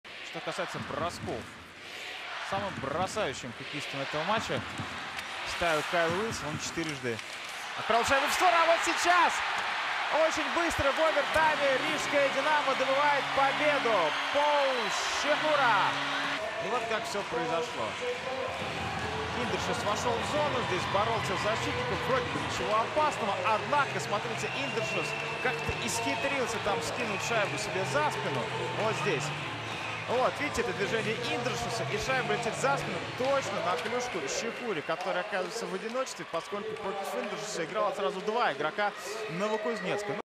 Слепой пас Индрашиса на победный гол Щехуры / Indrassist splendid blind pass on Szczechura GWG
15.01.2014, Микс Индрашис изловчился отдать слепой пас за спину на Пола Щехуру, который забросил победную шайбу в овертайме